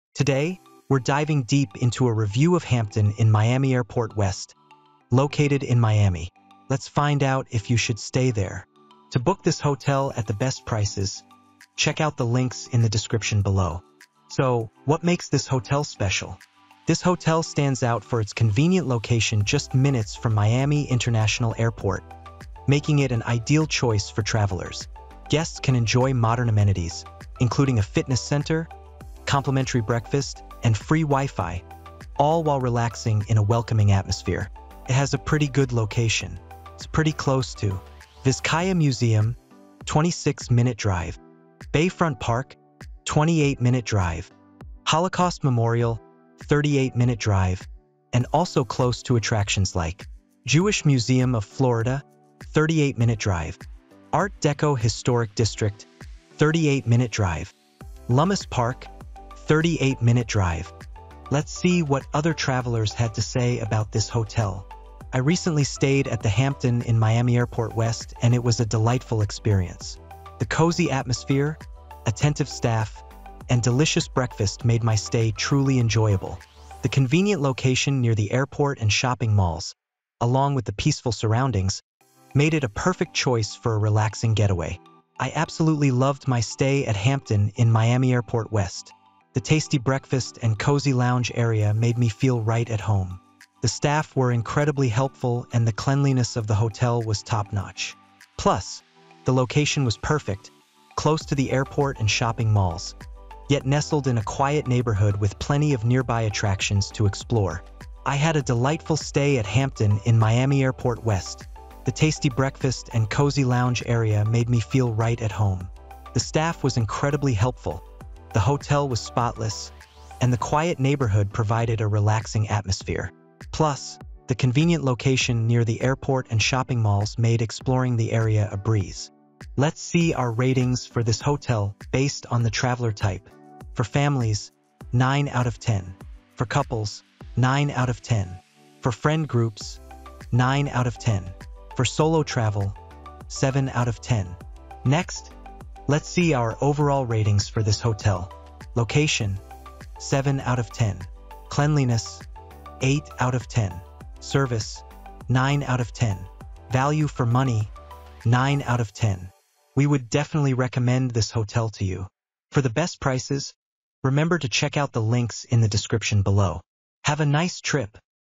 Hampton Inn Miami-Airport West Review - Is This Hotel Worth It?
In this video, we reviewed the Happy Life Grand Hotel & Sky Bar based on its price, quality, value for money, and more. We also gave it ratings based on the travel type and more.

Additionally, we'll provide you with insights into the nearby attractions and activities that you can enjoy during your stay at Happy Life Grand Hotel & Sky Bar. Whether it's exploring the local culture, sightseeing, or shopping, we've got you covered.